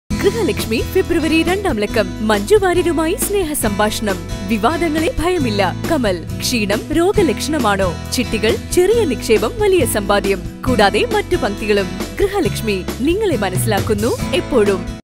Grihalakshmi February 2nd Issue Ad ft Vidya Balan, Minister Mercykuttyamma, Vidhubala and more
ഗൃഹലക്ഷ്‌മി ഫെബ്രുവരി രണ്ടാം ലക്കം വിപണിയിൽ ft Manju Warrier, Kamal and more!